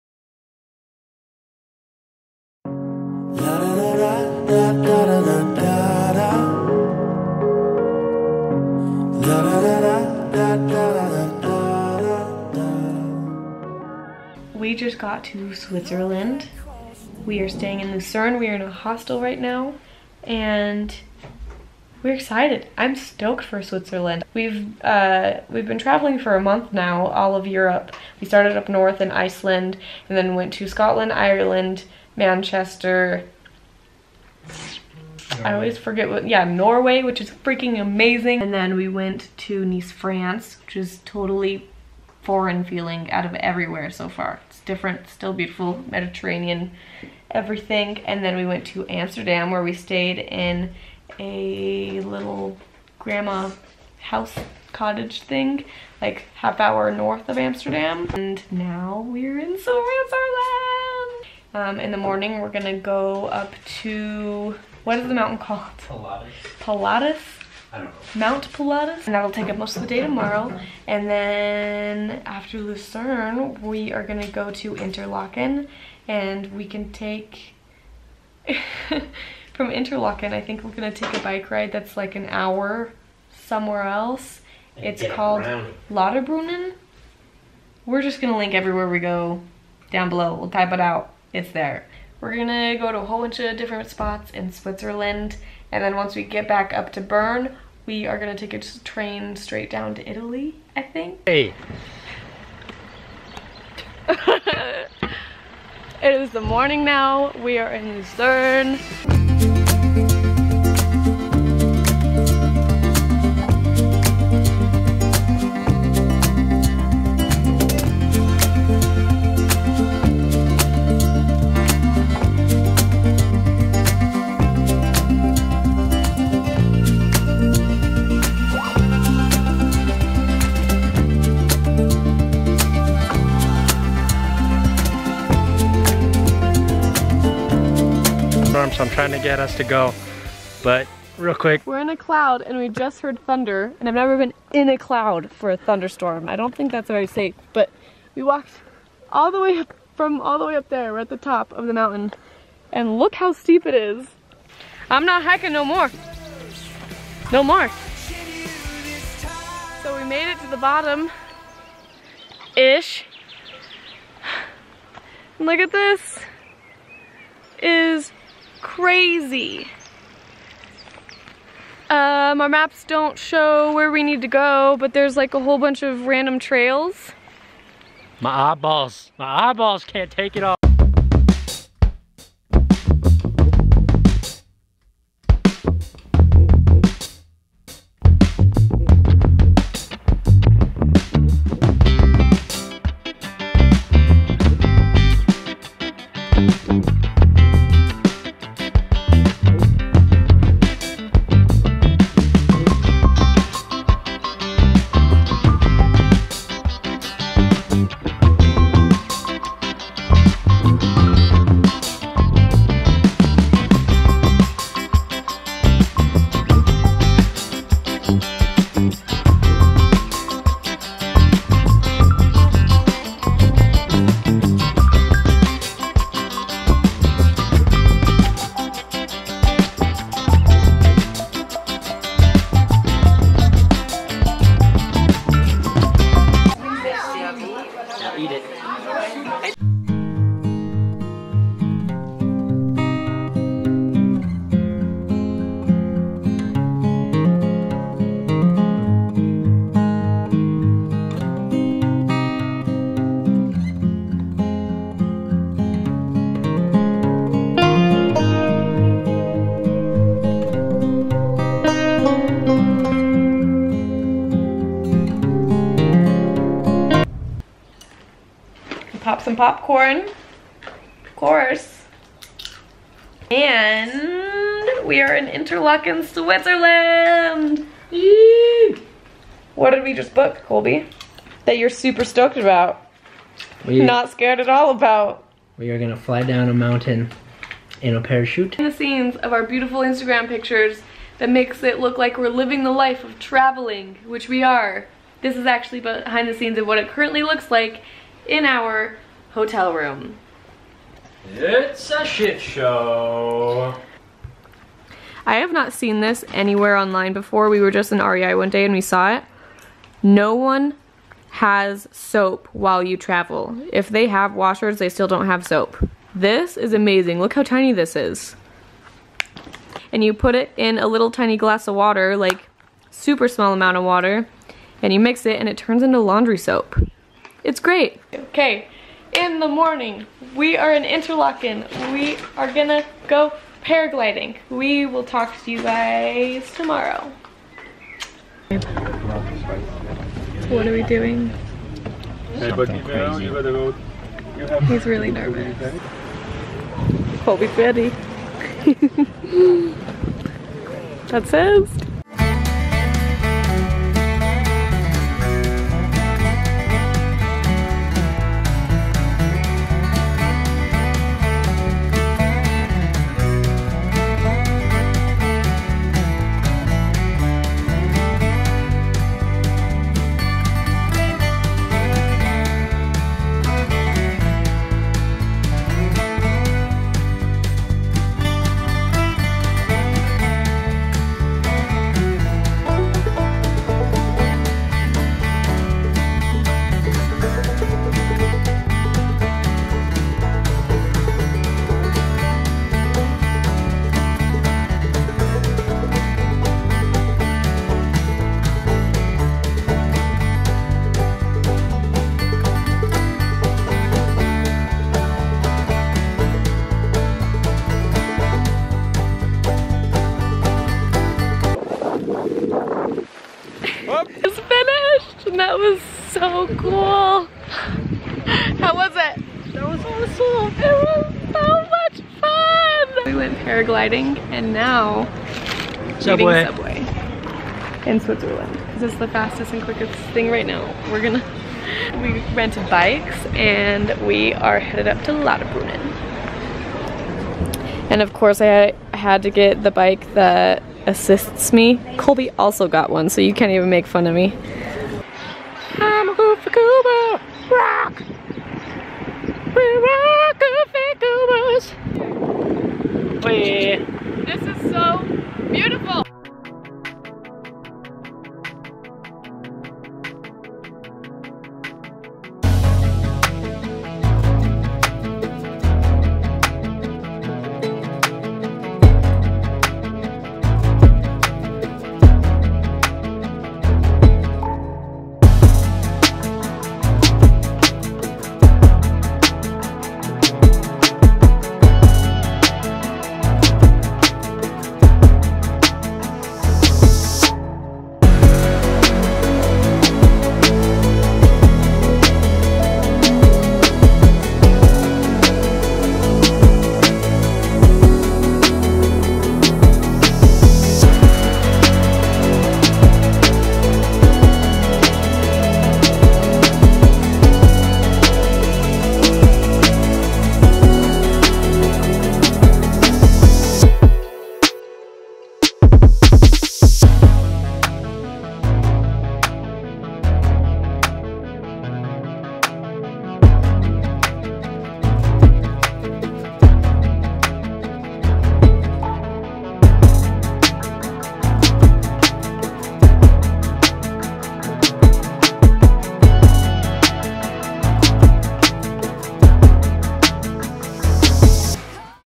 SWITZERLAND VLOG/Lost in a thunderstorm on top of a mountain?!
You are going to want to watch this vlog all the way through! We did sooo many exciting things in Switzerland. We got lost on a mountain and ended up hiking 5 hours in a thunderstorm, paraglided, rode bikes up to a small town on a beautiful day.

Switzerland literally was in our top 3 places on our Europe trip. I can easily say paragliding and then biking up to Lauterbrunnen was the BEST day of my life.

In this vlog we went to:
Interlaken
Gruyeres
Lucern
Mount Pilatus
Lauterbrunnen
Paragliding: Skywings Switzerland